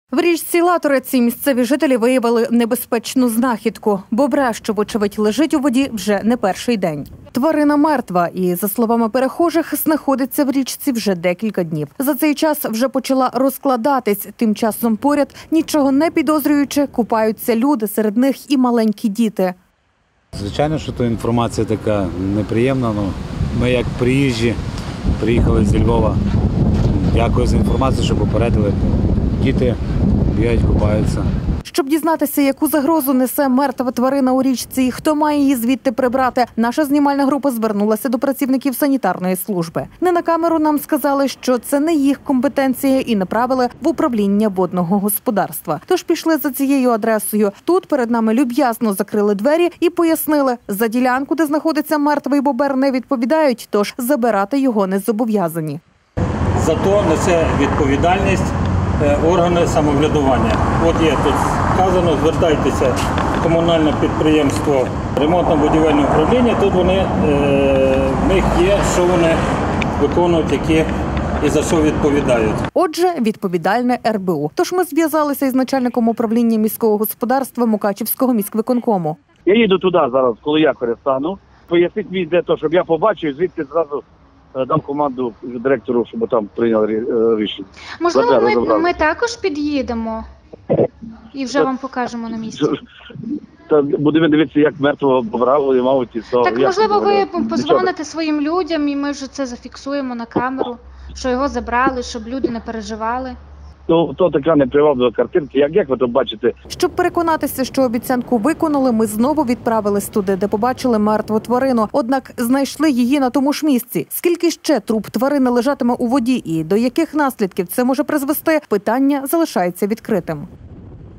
Небезпечна знахідка
Нікому це не заважає: у річці поруч із трупом купають дорослі та діти. Тварина мертва, й за словами перехожих, знаходиться в річці вже декілька днів. За цей час вже почала розкладатись, тим часом, поряд, нічого не підозрюючи, купаються люди, серед них і маленькі діти. Щоб дізнатися яку загрозу несе мертва тварина у річці і хто має її звідти прибрати, наша знімальна група звернулася до працівниківсанітарної служби. Не на камеру нам сказали, це не їх компетенція і направили в Управління водного господарства. Тож пішли за цією адресою. Тут перед нами «люб’язно» закрили двері й пояснили: за ділянку, де знаходиться мертвий бобер не відповідають, тож забирати його не зобов’язані. Отже – відповідальне РБУ. Тож ми зв’язалися із начальником Управляння Міського Господарства Мукачівського міськвиконкому. Щоб переконатися,що обіцянку виконали, ми знову відправились туди, де побачили мертву тварину. Однак знайшли її на тому ж місці.Скільки ще труп тварини лежатиме у воді і до яких наслідків це може призвести, питання залишається відкритим.